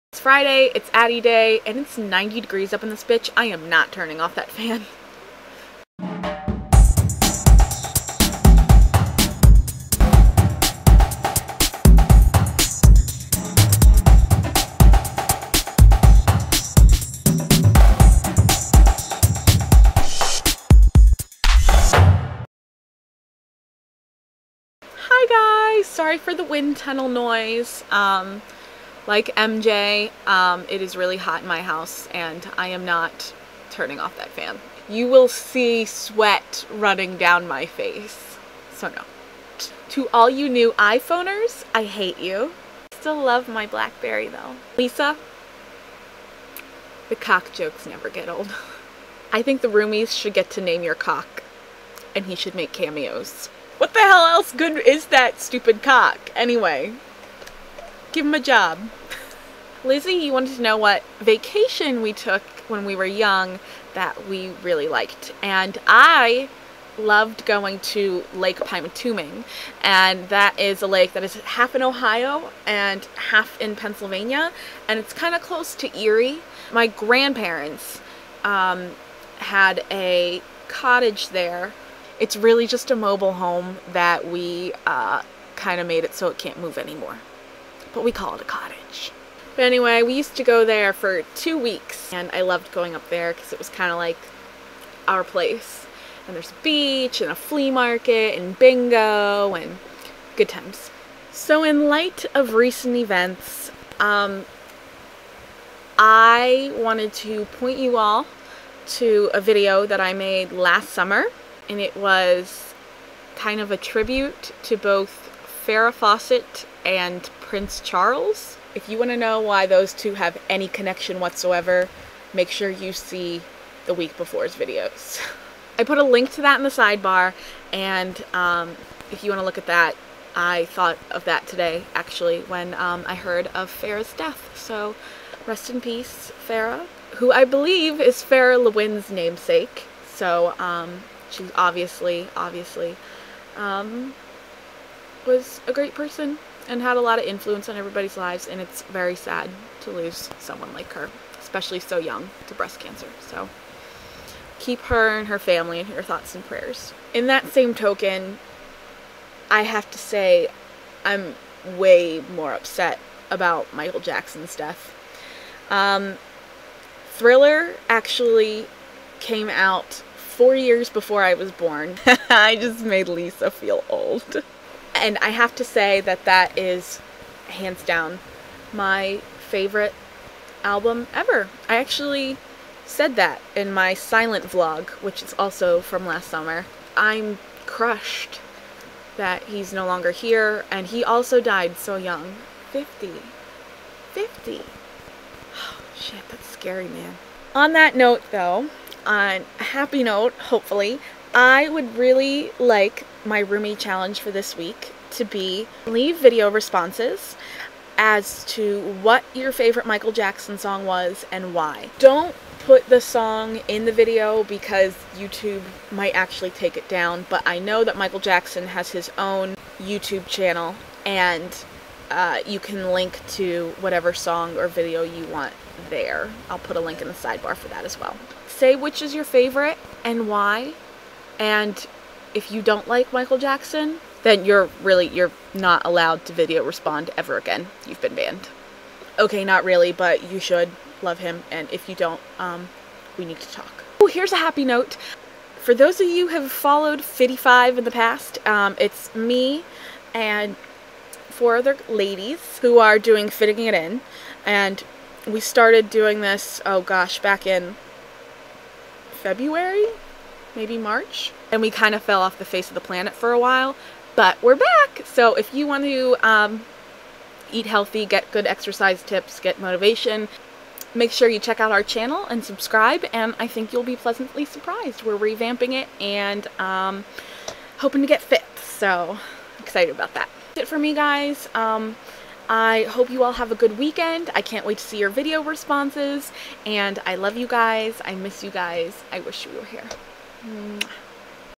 06.26.2009 - Addie says the cock jokes never get old
I made this and edited it all yesterday. I BET YOU FEEL LIKE YOU DONT EVEN KNOW ME ANYMORE!

in which Addie forgets to answer her own questions, gives you lots of links, and DOES NOT GO OVER 5 MINUTES! WOOOOO!!

Roomie Challenge: fav Michael Jackson song